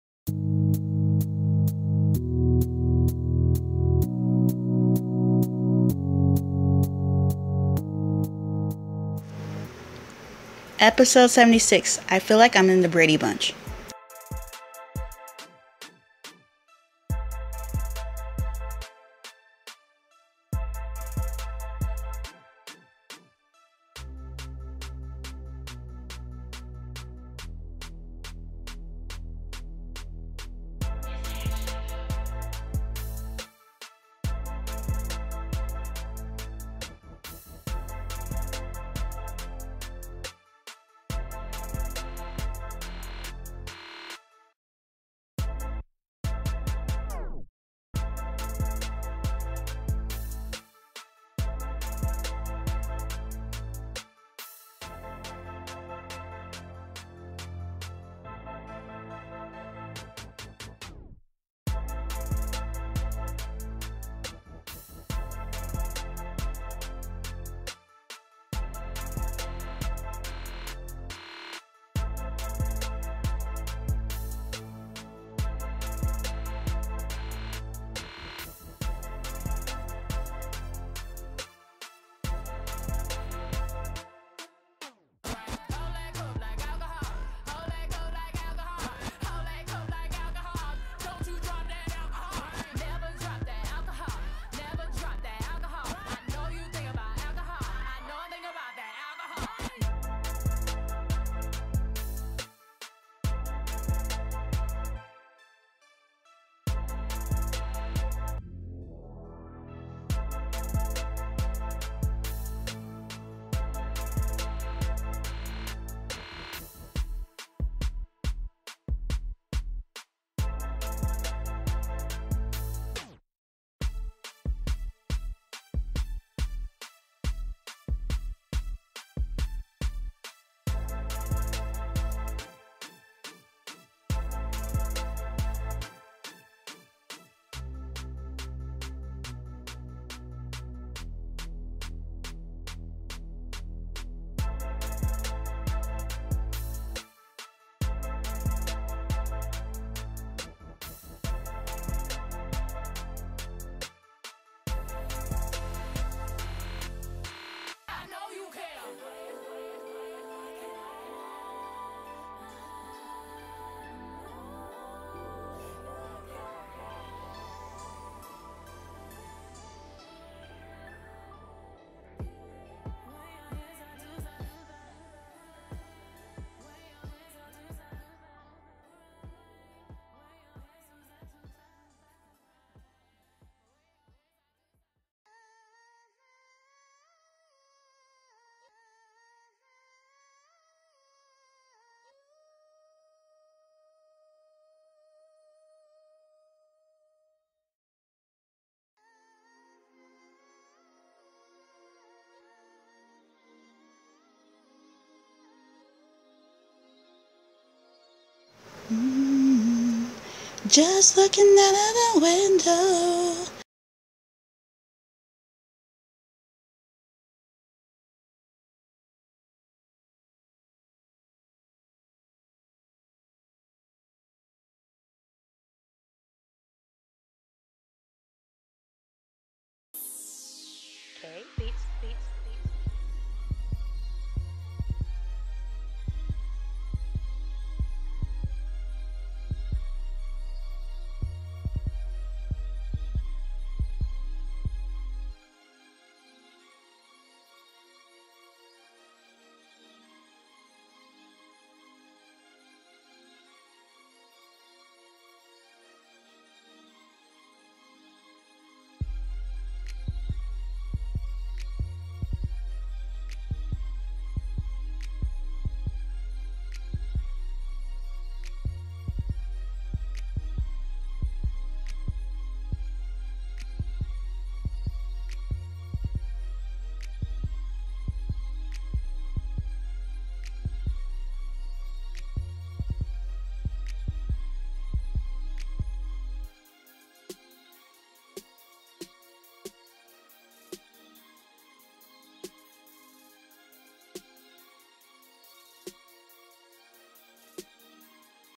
Episode 76:I Feel Like I'm In The Brady Bunch
Welcome to another episode of Larissa and Her 100 Babies , this episode had a lot of stars though their appearances were brief. Oh and sorry about the quality saved it in hd and doesn't look right at all.
 and my second twitter and main one for youtube MsSimette